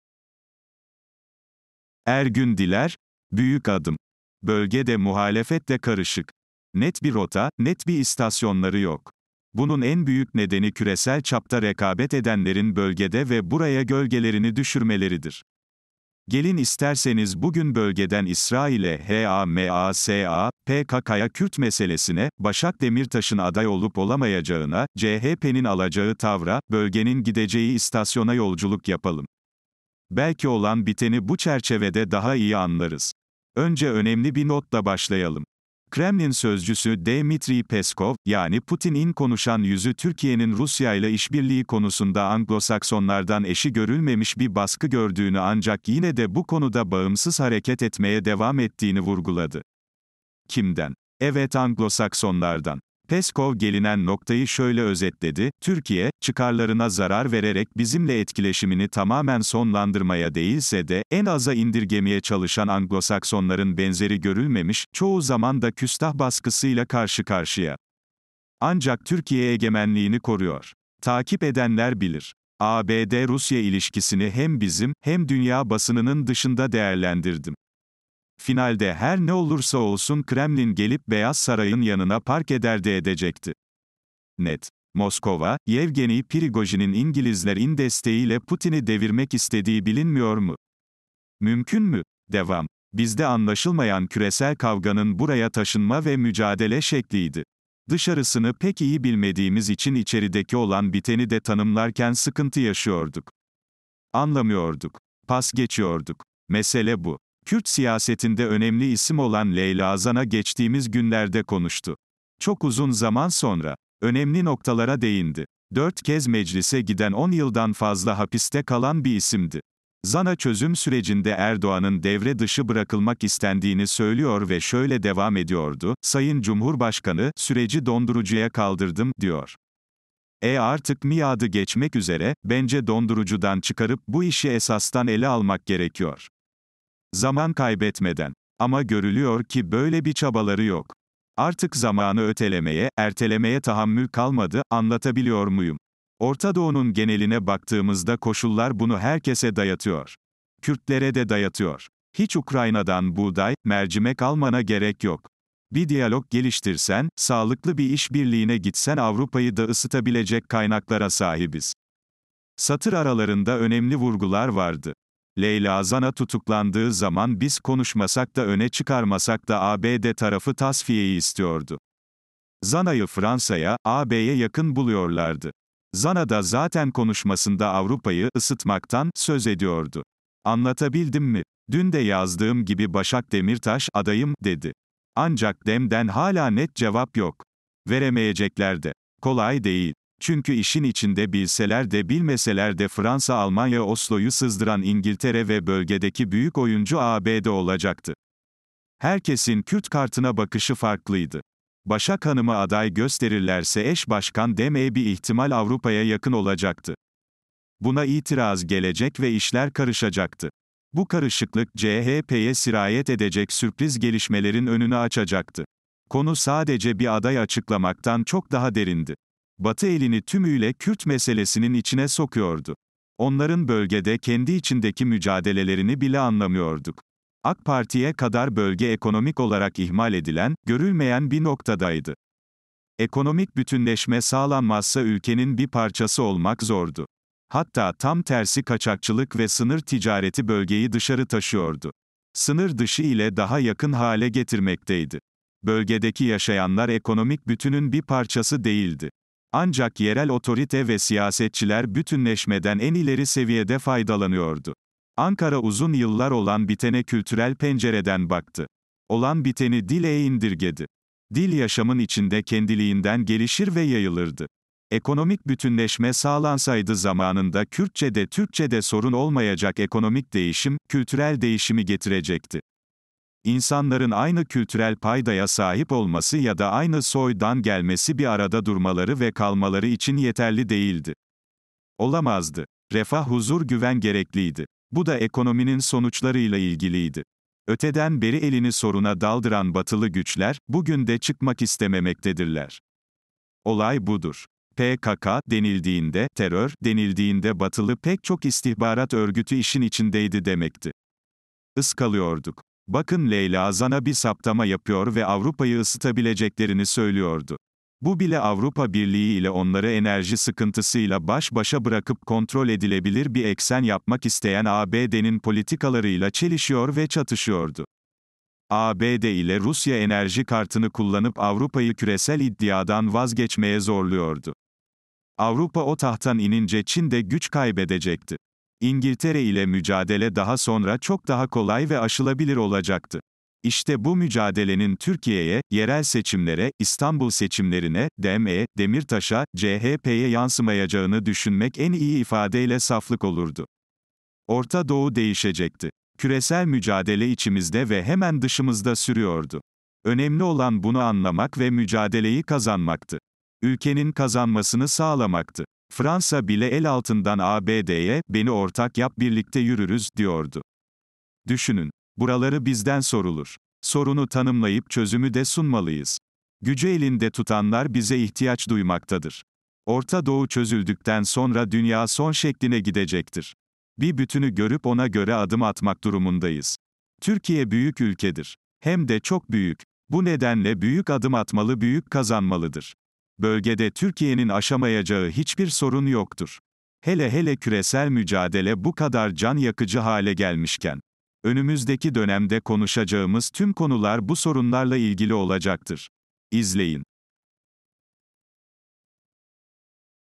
ERGÜN DİLER: BÜYÜK ADIM - SESLİ MAKALE DİNLE
Ergün Diler - ahaber.com - Büyük adım - 3 Şubat 2024 tarihli yazısının seslendirmesi

"BÖLGE de muhalefet de karışık. Net bir rota, net bir istasyonları yok. Bunun en büyük nedeni KÜRESEL ÇAPTA REKABET EDENLERİN bölgede ve buraya gölgelerini düşürmeleridir. Gelin isterseniz bugün bölgeden İsrail'e HAMAS'a, PKK'ya Kürt meselesine, Başak Demirtaş'ın aday olup olamayacağına, CHP'nin alacağı tavra, bölgenin gideceği istasyona yolculuk yapalım.
 Belki olan biteni bu çerçevede daha iyi anlarız.
 Önce önemli bir notla başlayalım...
Kremlin Sözcüsü Dmitriy Peskov, yani PUTİN'in konuşan y. "